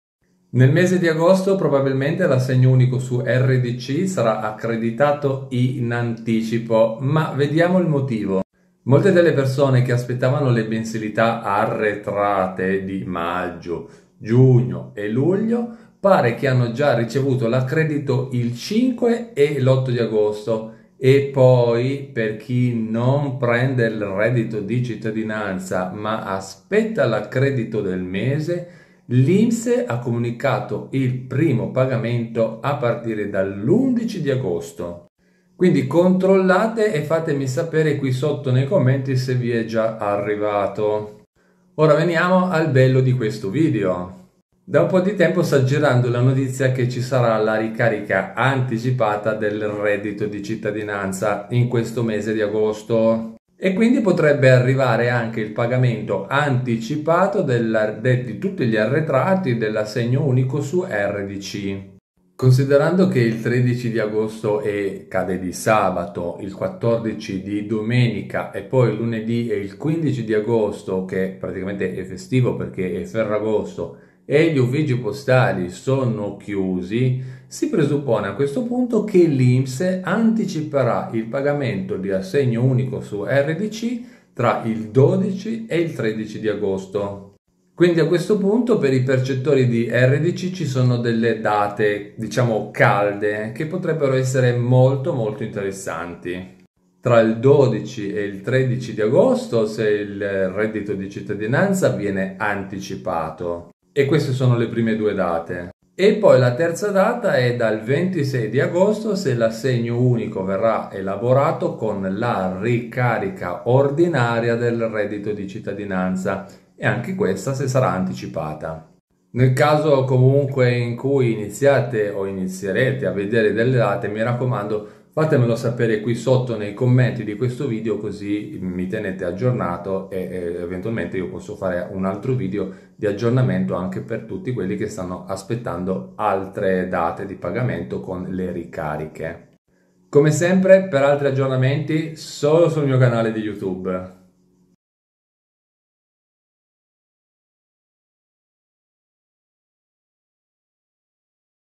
AU su Rdc Anticipati Con Nuove Date “Calde”
E' tanto tempo che vi porto novità e notizie
Se volete supportare il mio canale, potete offrire un caffe qui

Nel mese di agosto, probabilmente l’assegno unico su Rdc sarà accreditato in anticipo, ma vediamo il motivo.

Molte delle persone che aspettavano le mensilità arretrate di maggio, giugno e luglio pare che hanno già ricevuto l'accredito il 5 e l’8 agosto e poi per chi NON prende il RDC , ma aspetta l'accredito del mese l’INPS ha comunicato il primo pagamento all'11 agosto.

Quindi controllate e fatemi sapere qui nei commenti se vi è arrivato.

Ora veniamo al bello del video...
Dopo che sta girando la notizia che ci sarà la ricarica anticipata del reddito di cittadinanza potrebbe arrivare anche il pagamento anticipato degli arretrati dell’assegno unico su Rdc.

in merito alle pubblicazioni del Rag. Melacarne